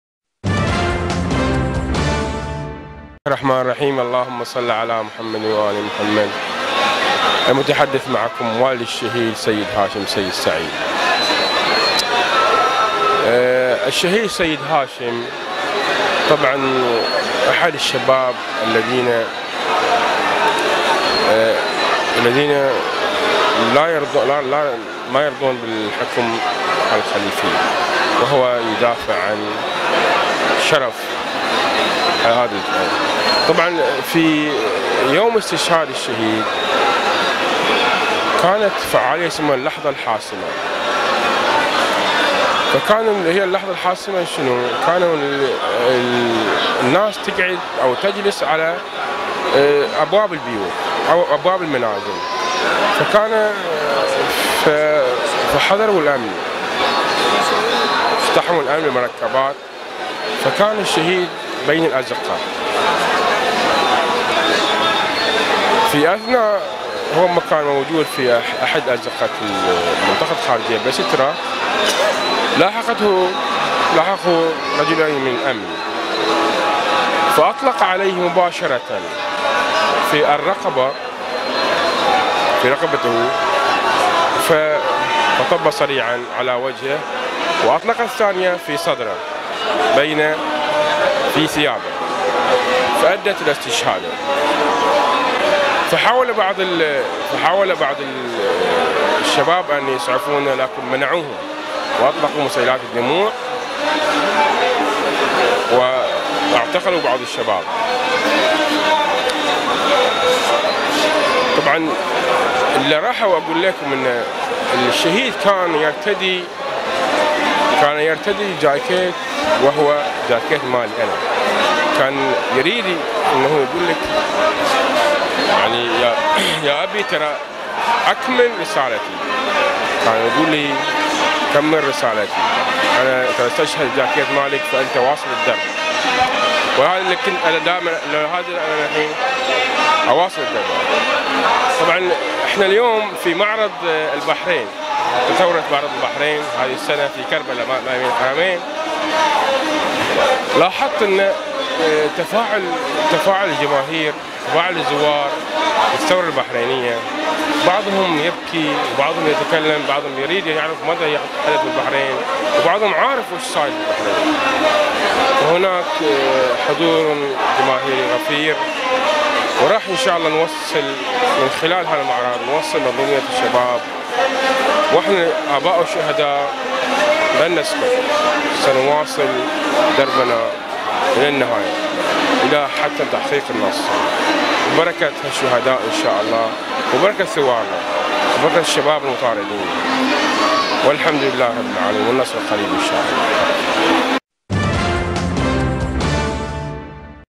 والد الشهيد سيدهاشم يعرف بإبنه في معرض الثورة البحرينية في كربلاء المقدسة
والد الشهيد سيدهاشم سعيد يعرف بإينه الشهيد سيدهاشم في معرض الثورة البحرينية الذي اقيم في مدينة كربلاء المقدسة بالعراق في أيام زيارة الأربعين 1435 هـ , حيث يذكر بأن الشهيد سيد هاشم قد استشهد بعد قمع قوات النظام الخليفي فعالية اللحضة الحاسمة في جزيرة سترة جنوب العاصمة المنامة